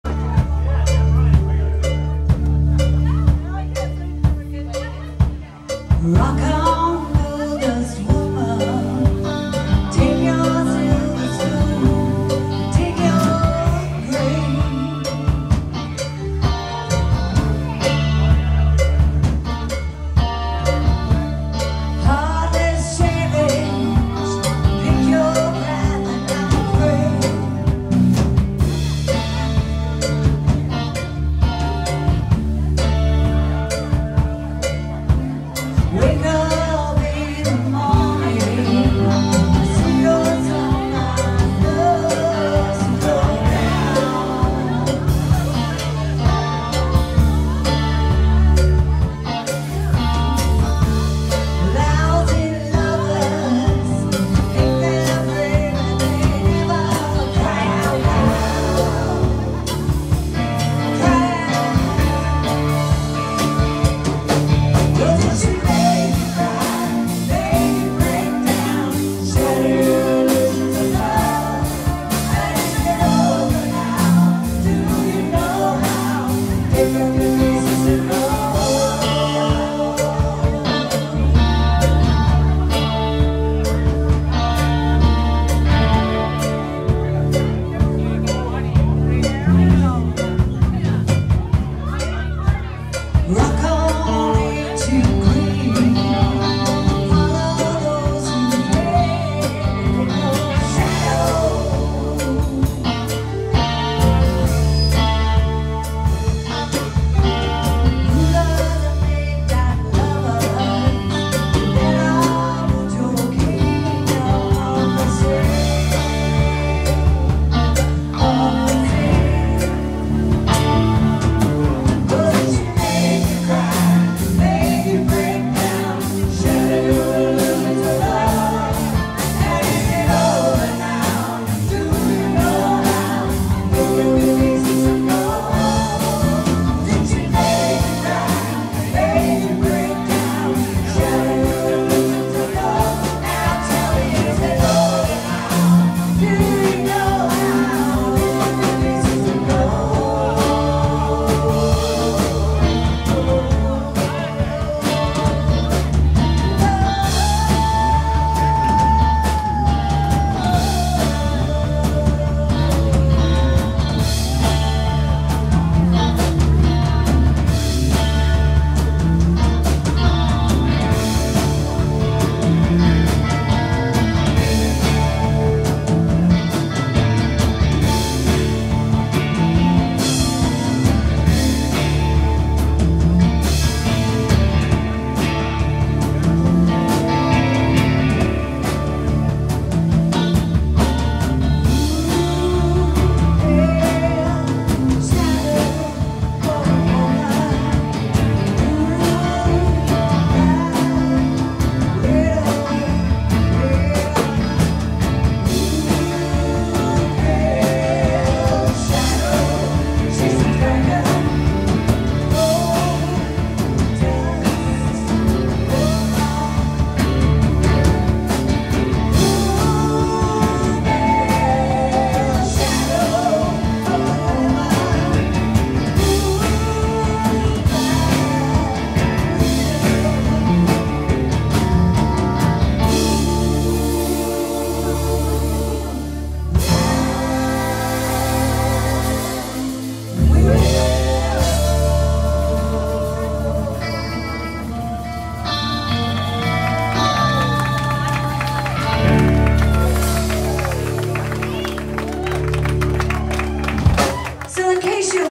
Second Hand Newz Dec 13 2013 (3)
Second Hand Newz (Fleetwood Mac tribute) at One-Eyed Jack's Roadhouse in Lynnwood WA